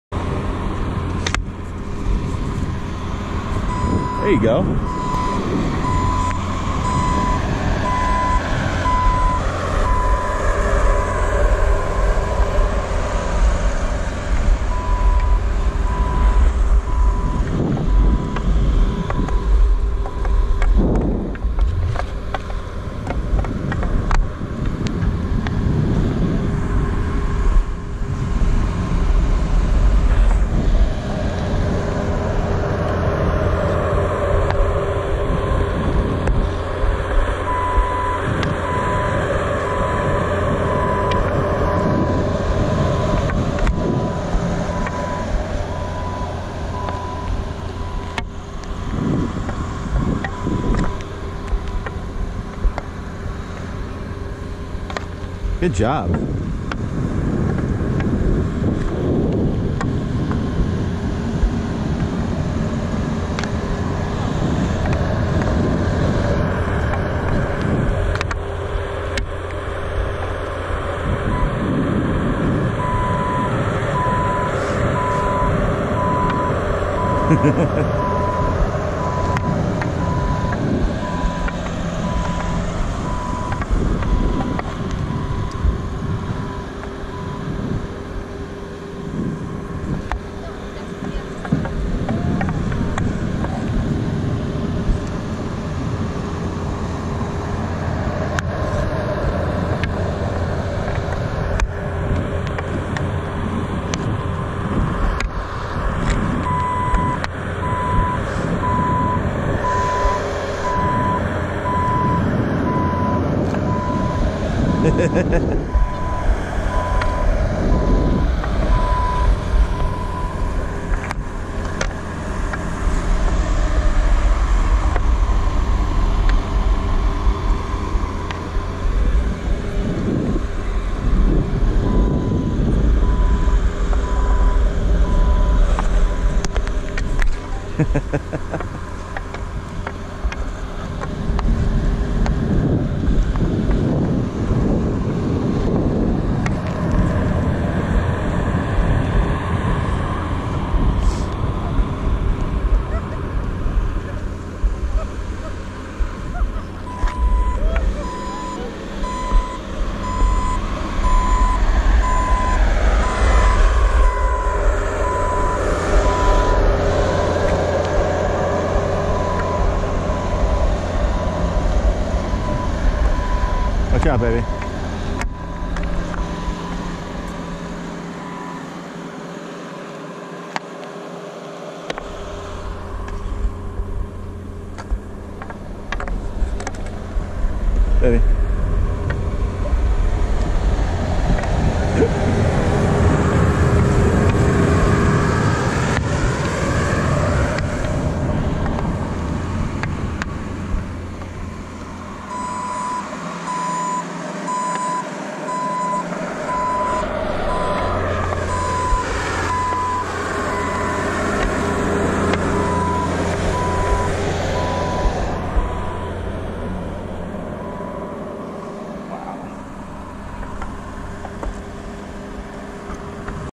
Pond Hockey on Lake Louise
Skating the pond, playing a little pond hockey